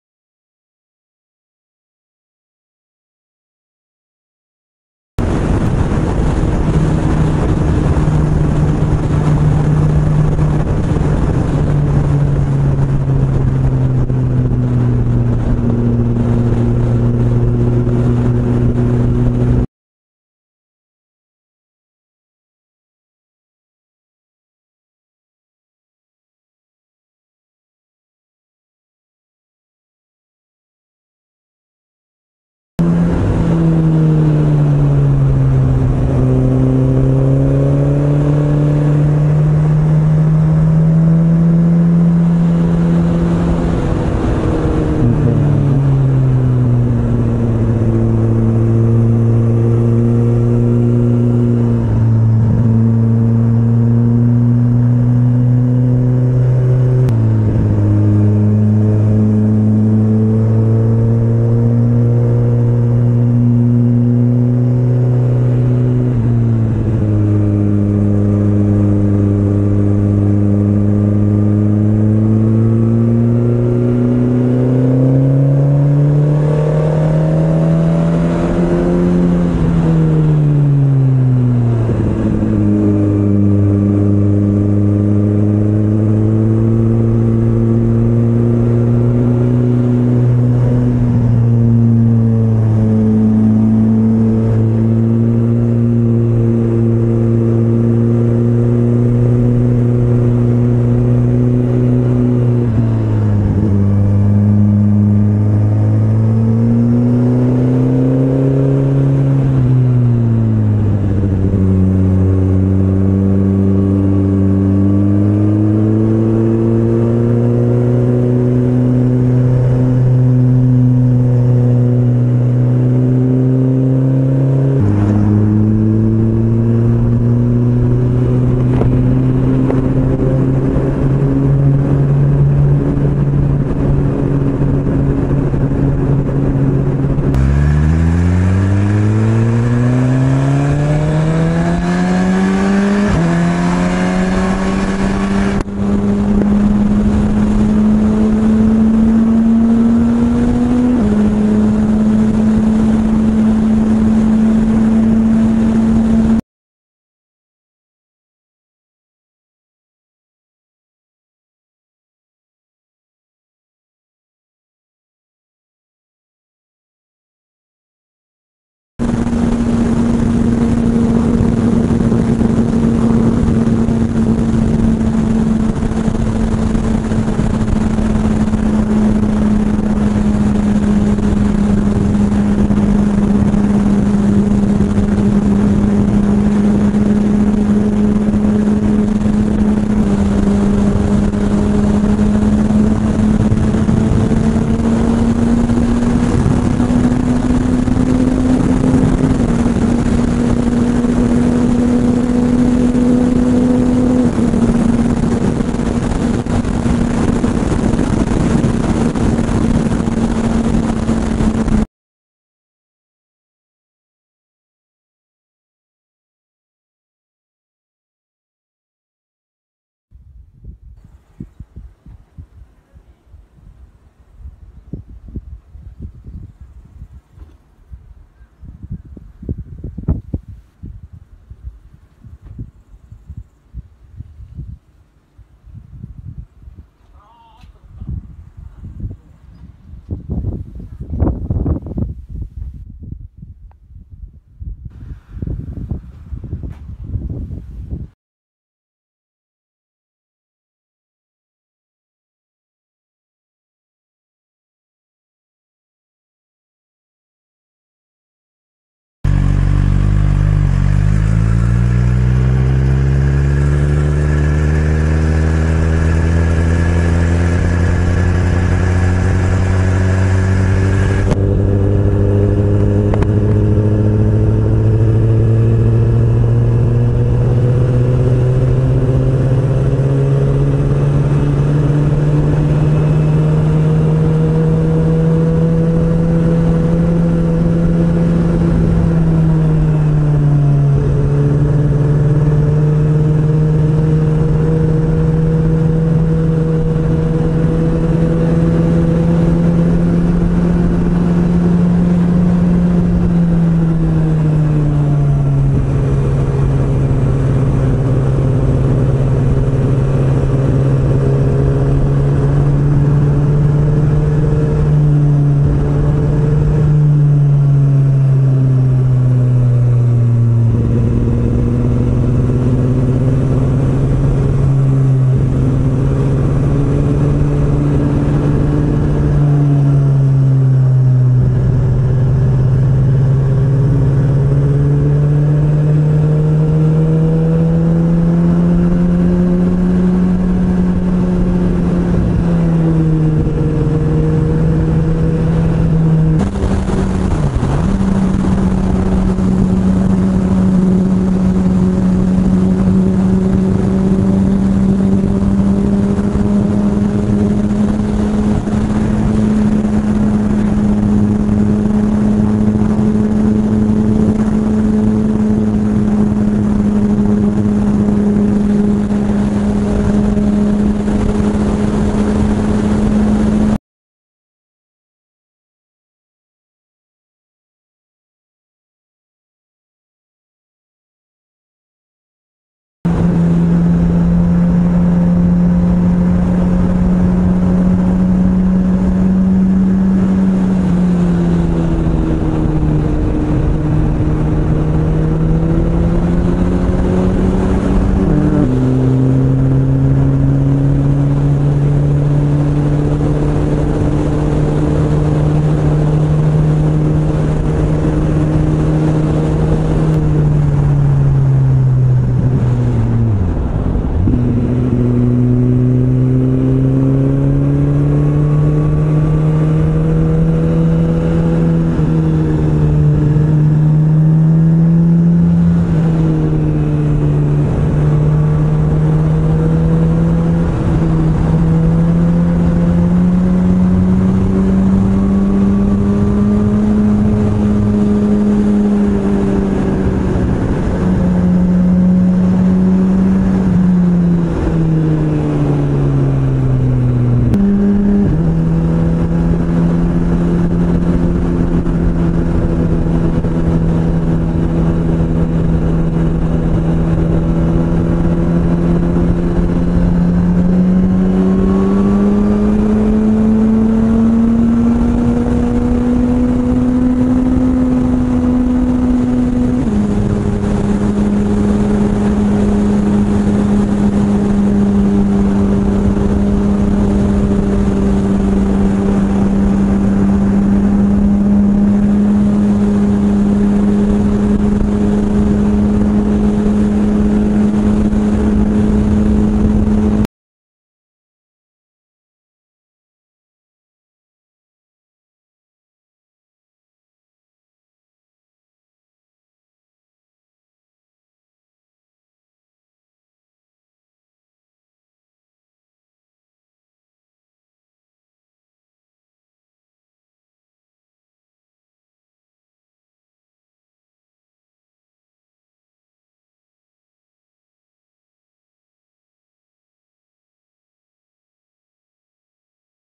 2018 07 13 GSX R1000R 주행영상
김해-삼랑진-경주 토함산-영산-김해

주행시간 5h 20m

SUZUKI; GSX-R1000R L7
SONY; HDR-AS300